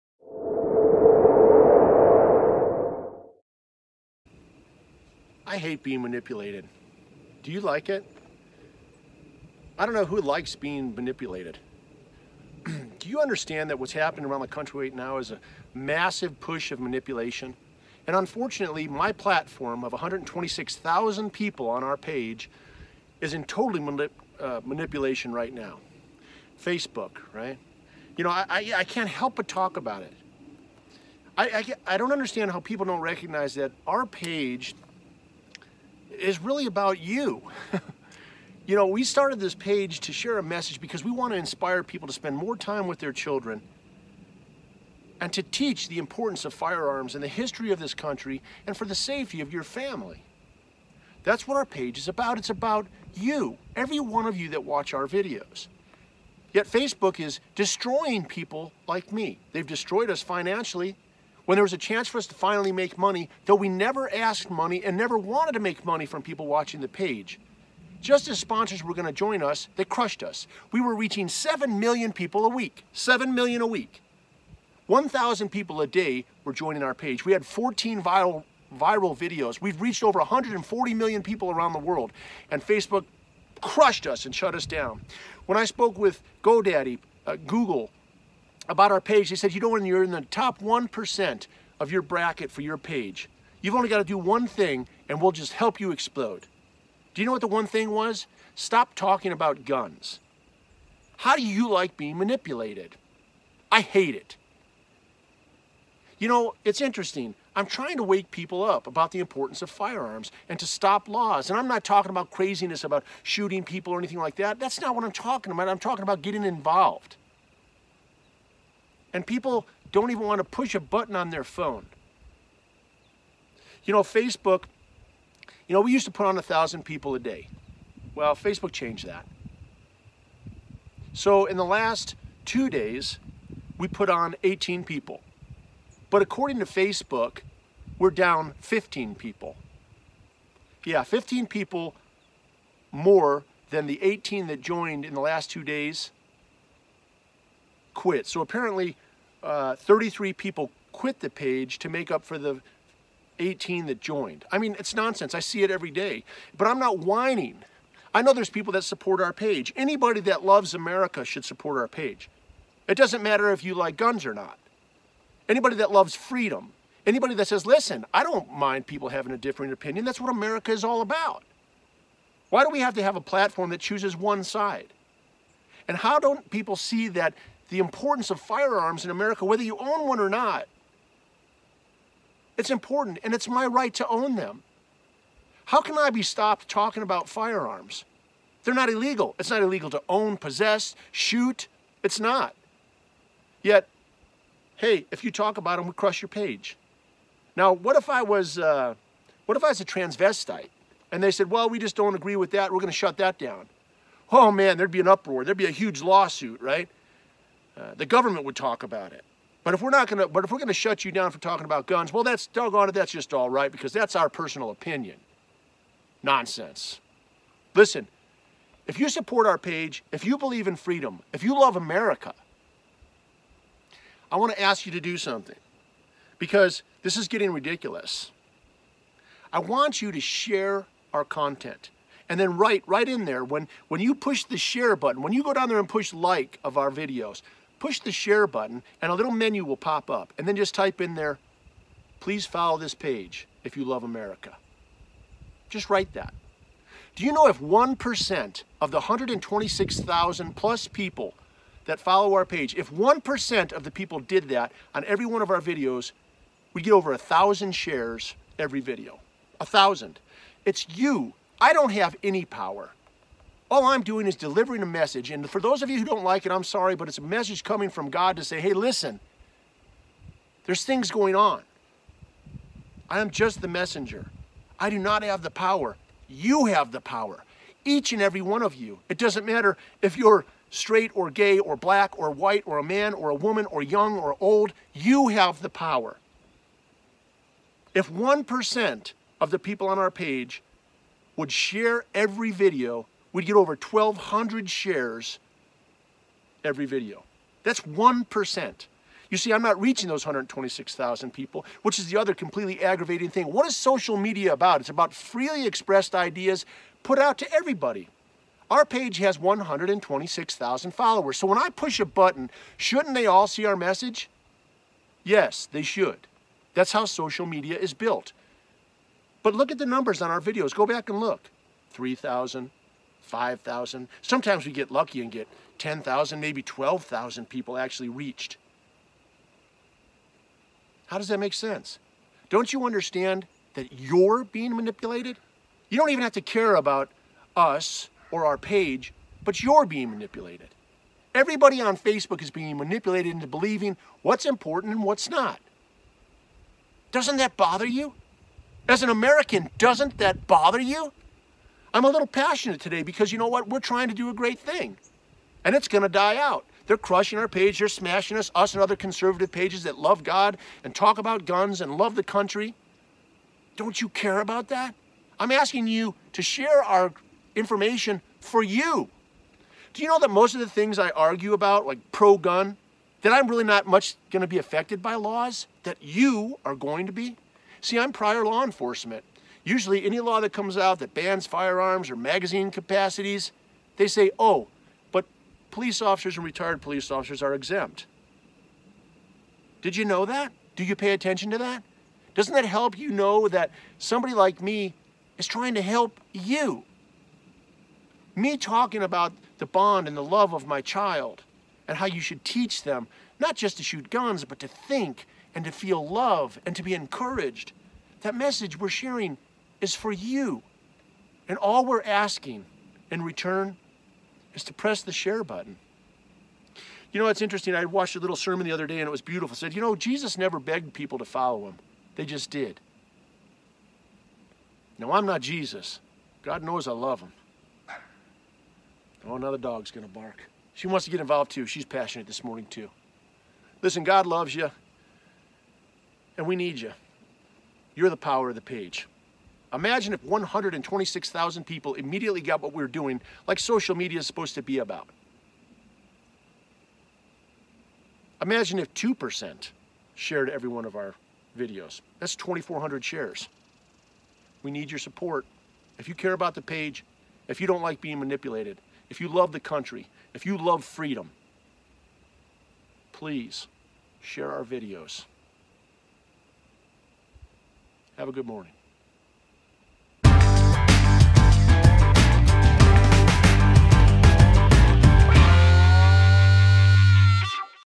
Coffee with Vantac (19)
The power of Vantac is "YOU"

Manipulation of the truth is not truth, it is manipulation.

Over 2.3 billion people a month utilize FaceBook,. Yes:  2,300,000,000 people utilize FaceBook monthly.

How important is it that a public forum of that size share the truth?
Especially when their proclaimed business motto is the public sharing of free information.

Here is an example:
What if only one side of abortion was shared on a platform of that magnitude?  Only the (seemingly) positves of abortion, population control in starving countries, deformed and sickly children eliminated, rape victims, a woman's choice over her body.
No discussion of the right to life;  that children are humans in the womb; the precious and innocent love demonstrated by children.

Did you see the photo of the premature baby still in the placenta, reaching its hand out and gripping the doctor's finger? Look up the photo. Anyone not affected by that photo is simply not human.

The elimination of one side of a two sided issue is manipulation. It robs all those who care to make an educated decision on an issue of the truth.
Thus, manipulation is an untruth, a lie, an injustice, inappropriate, unethical and should simply not be acceptable to a morally sound populace.

What will it take for those (that I believe to be a majority) in the population to stop this type of behavior?
Will it be when all dialogue is stopped?
When all those with dissenting opinions are removed?
When the (proverbial) water is boiling in the pan containing the frog?

We hope that enough of our supporters hope to draw the line much earlier. That truth is important; that freedom means just what is says.

As always, we appreciate your support.  Your support is essential in the future of your life, your country, your freedoms.

We hope we have earned your trust.